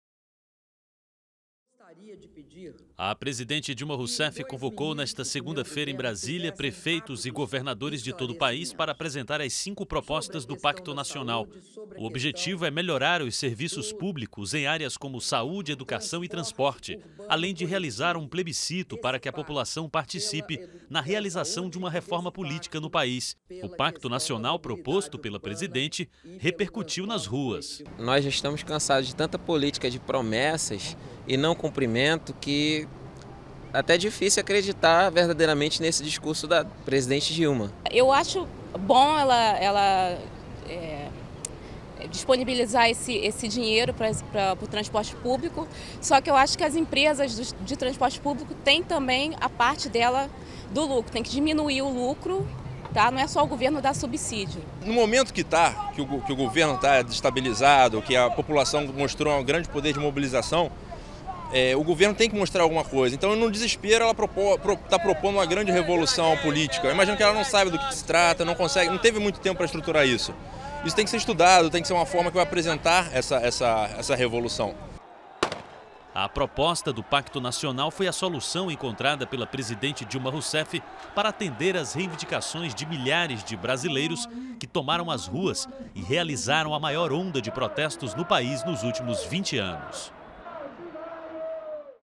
Brasileiros avaliam propostas de Dilma
As propostas apresentadas pela presidente Dilma Rousseff, nesta segunda-feira, repercutiram pelo país. Durante o encontro com prefeitos e governadores, Dilma propôs a elaboração de um pacto nacional para melhorar saúde e educação, além de um plebiscito sobre a reforma política.